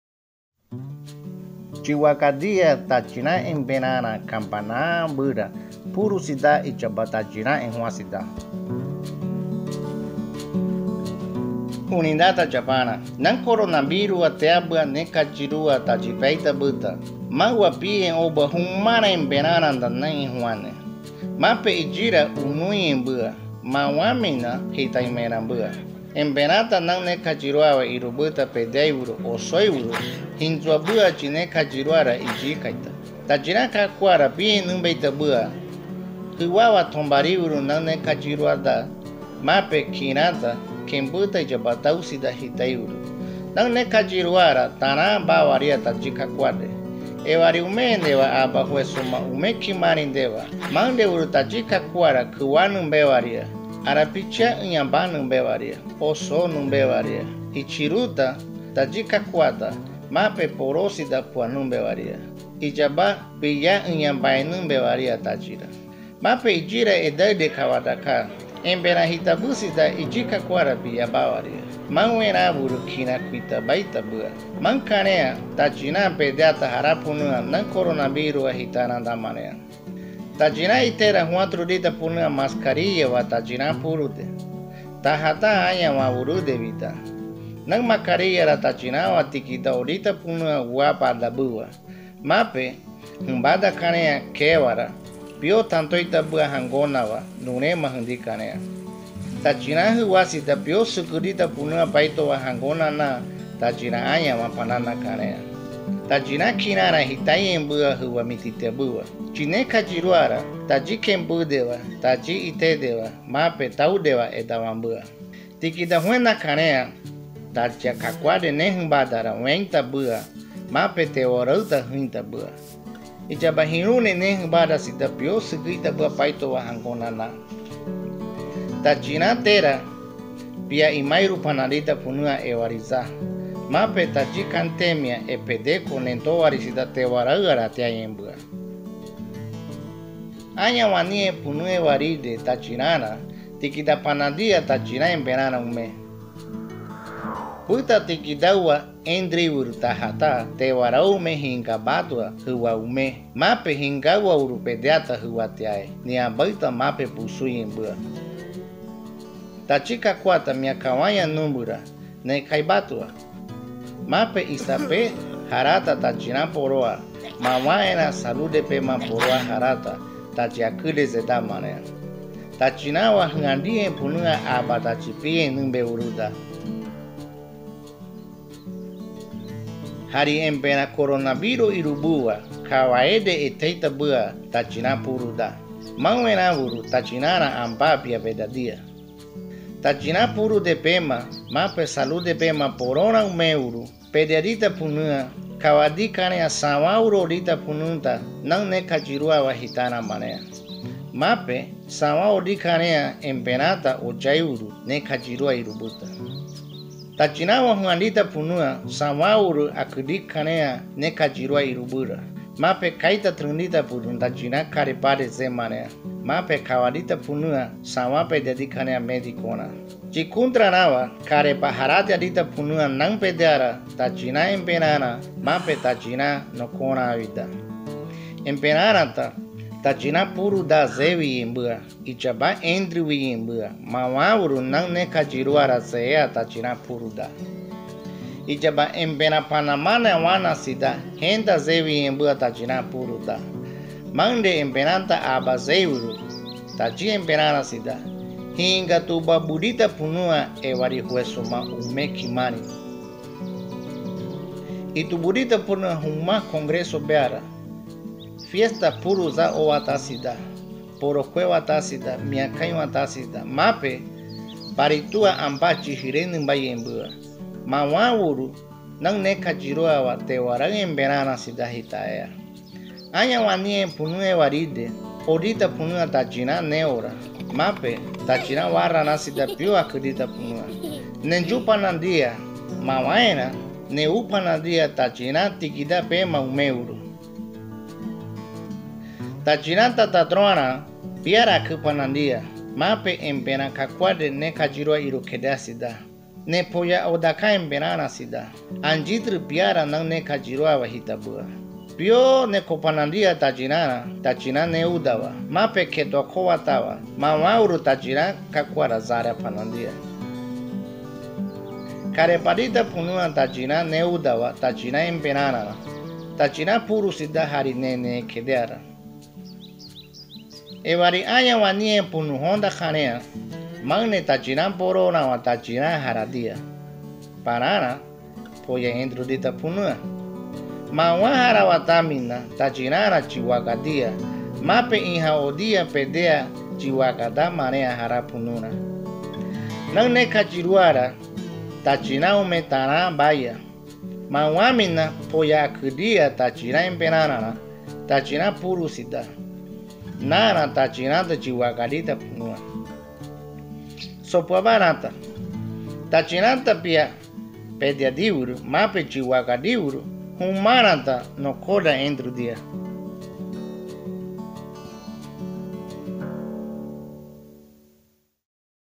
Cuidémonos en la pandemia. BID_MINSA Panamá (Emberá)
Video animaciones que explican de manera didáctica las medidas de prevención del COVID-19 en lenguas originarias de los pueblos indígenas de América Latina.